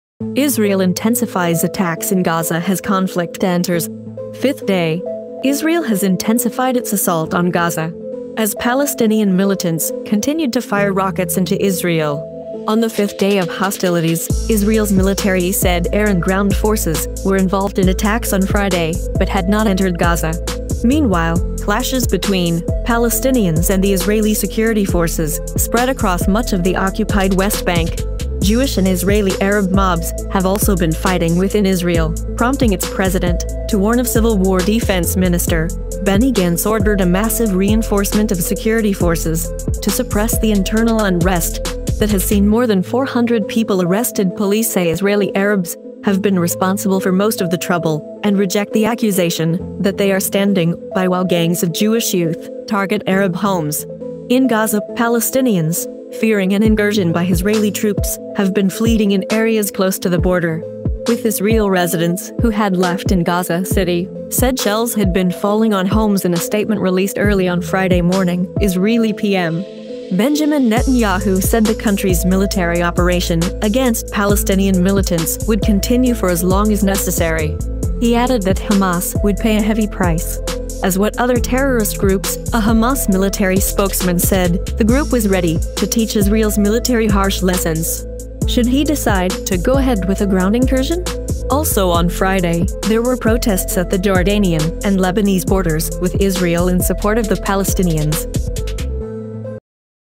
Israel prepare the Attacks In Gaza As Conflict Enters Fifth Day | FastNews
IMPORTANT!